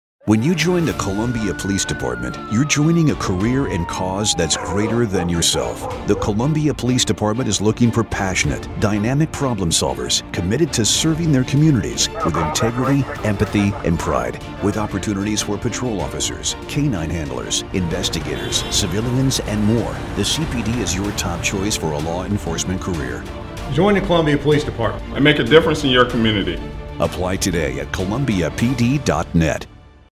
Join the Columbia Police Department Today!
The Columbia Police Department is now hiring!

Join today and receive a hiring bonus up to $5,000 for certified officers!